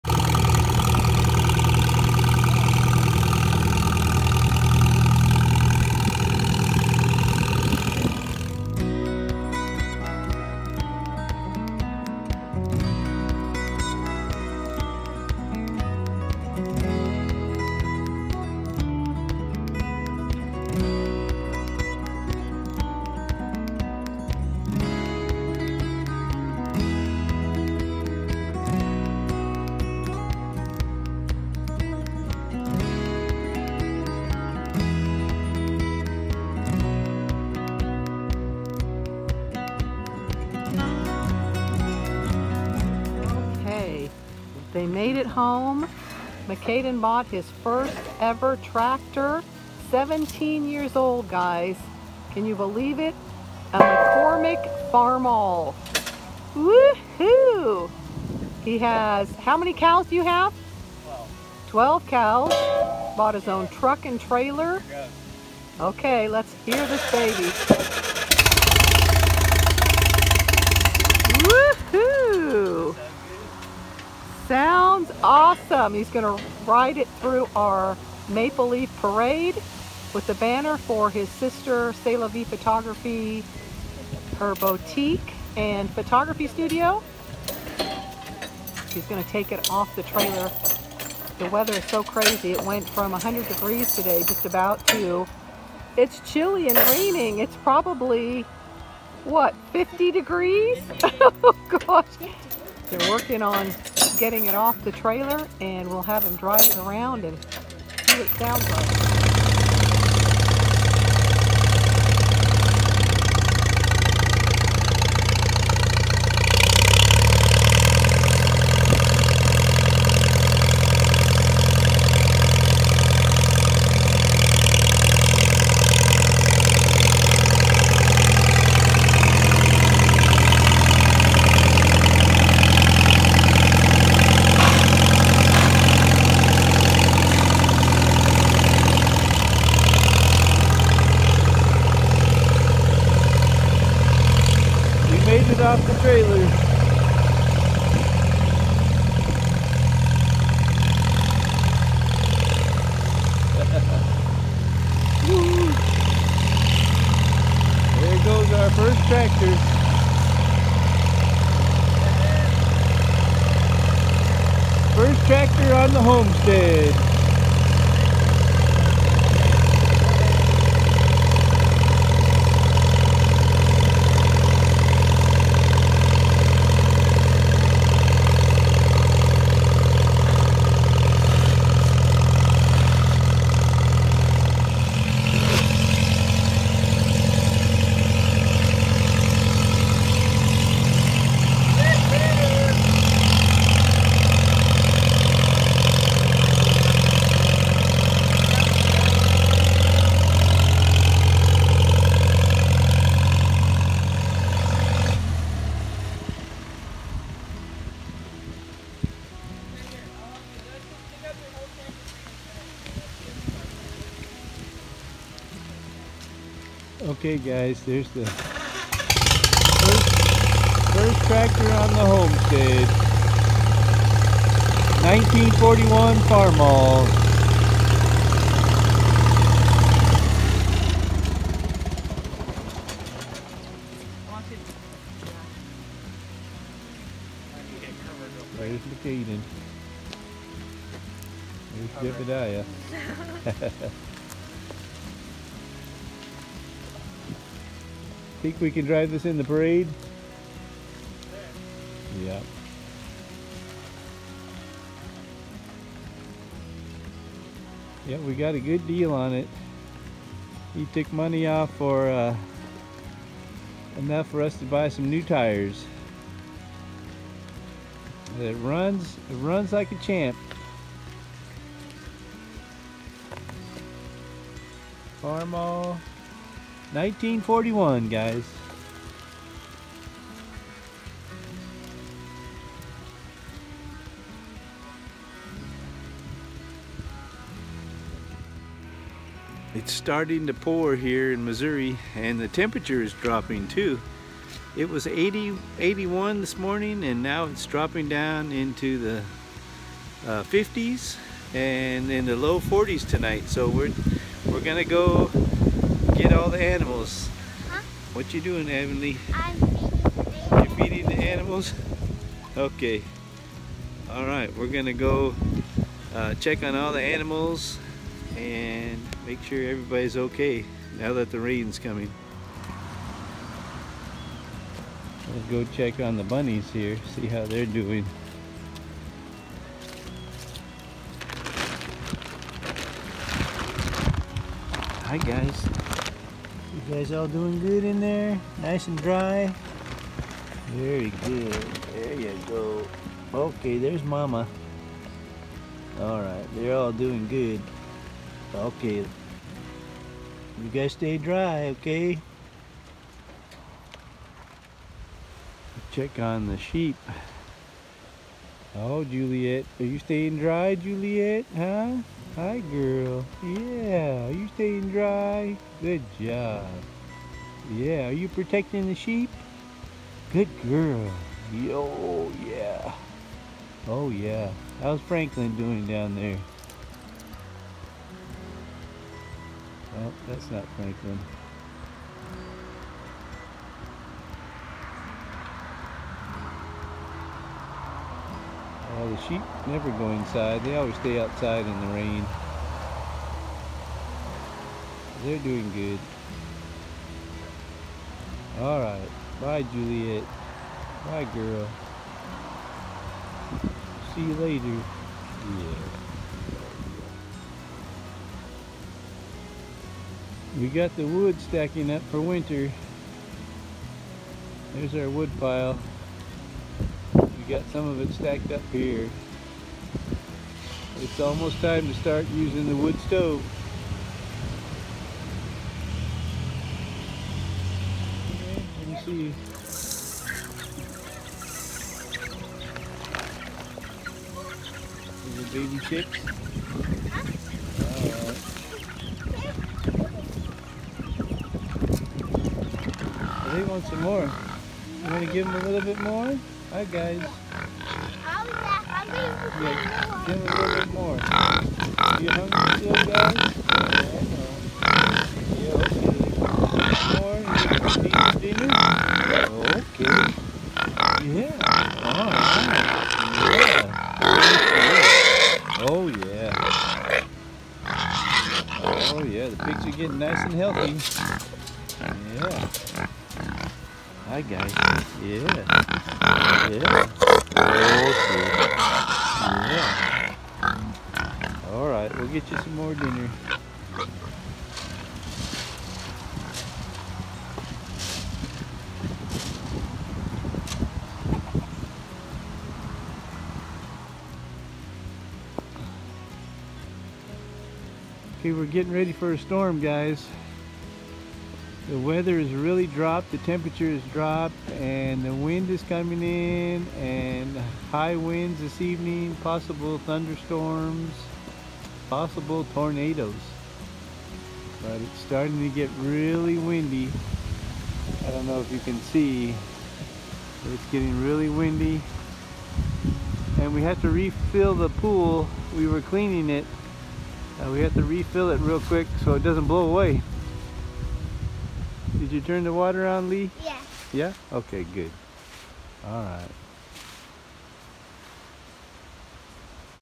FARMALL H 1946/ New On The Homestead
Mackaiden bought his first ever tractor a 1941 Farmall, it’s fire engine red, such a gem, it’s been restored back to it original beauty. It was quite a job to get this huge beast home in the rain but they did it. The animals are such a blessing on the homestead.